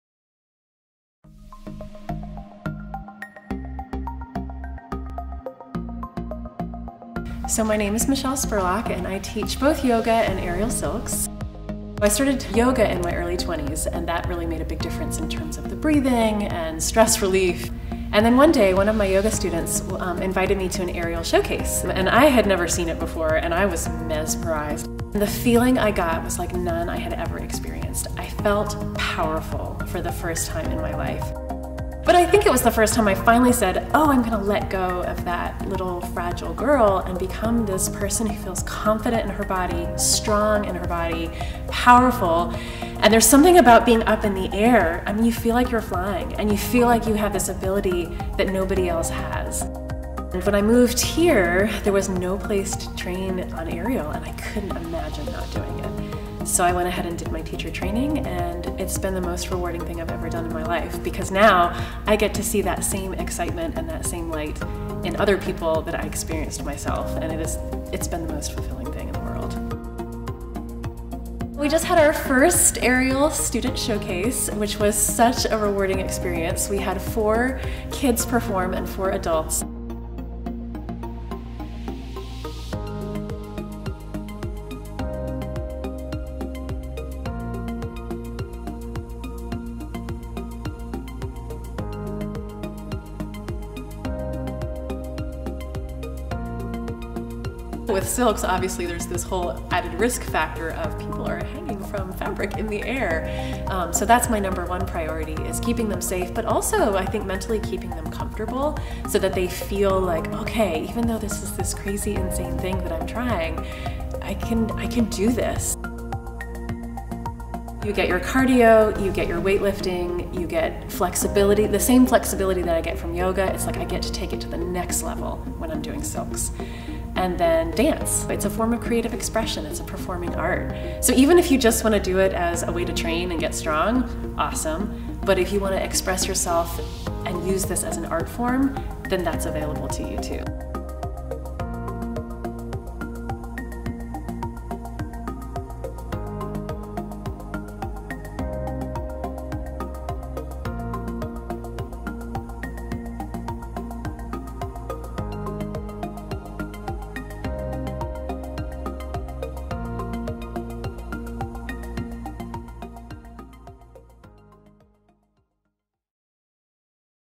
Aerial Silks Classes with Michelle Spurlock
Interested in trying Aerial Silks? Check out this video with aerial silks instructor, Michelle Spurlock

Video Credit: Rhett Lawson
ETV Advanced Equipment Specialist
Technology and Learning Services
Elon University Class of 2017
B.F.A. Cinema and Television Arts
Entrepreneurship Studies